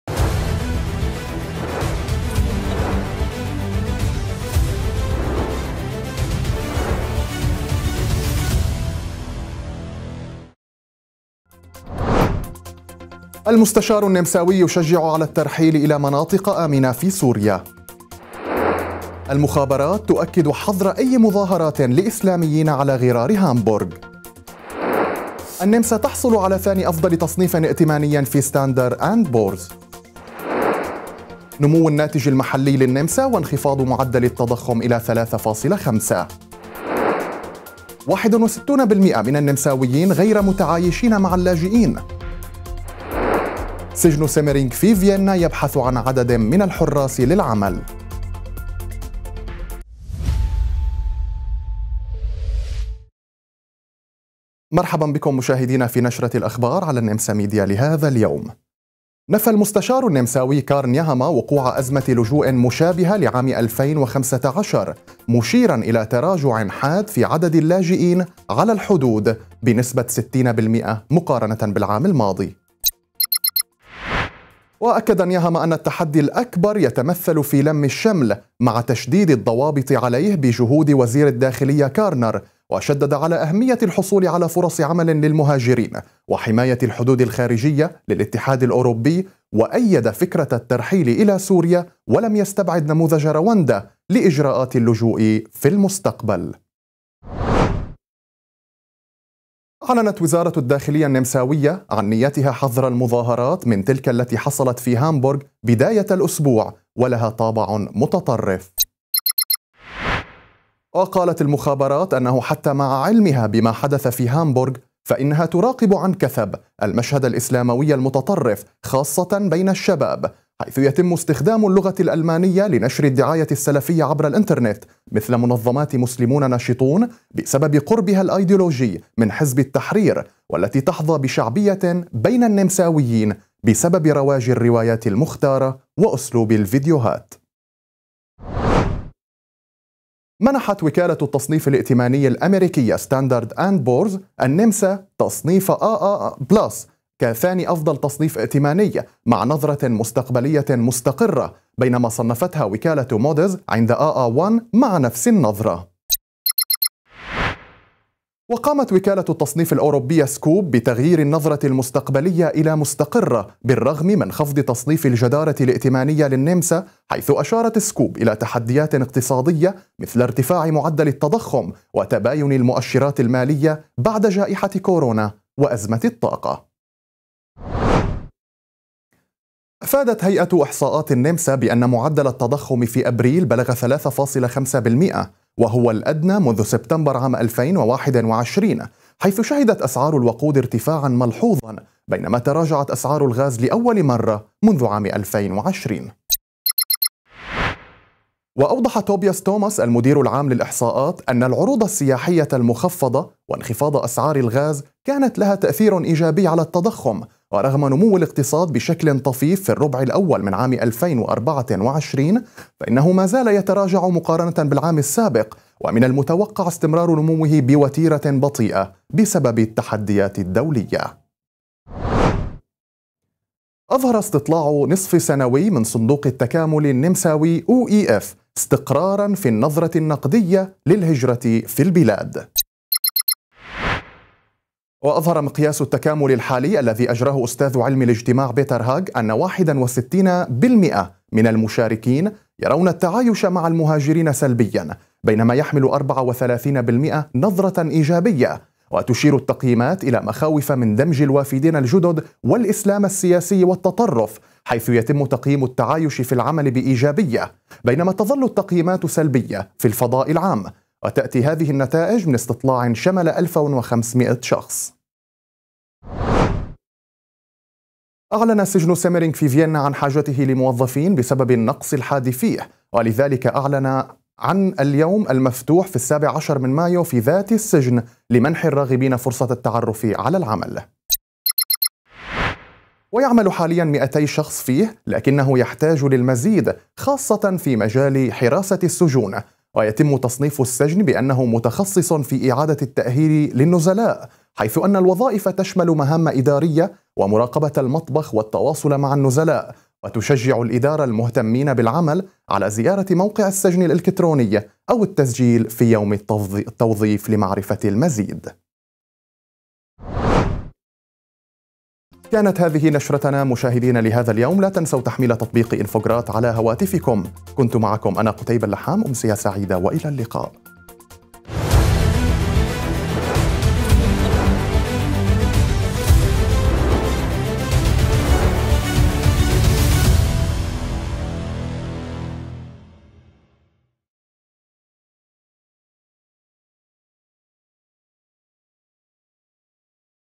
المستشار يشجع على الترحيل الى "مناطق آمنة في سوريا" و61 % من النمساويين غير متعايشين مع اللاجئين
تقديم: قتيبة اللحام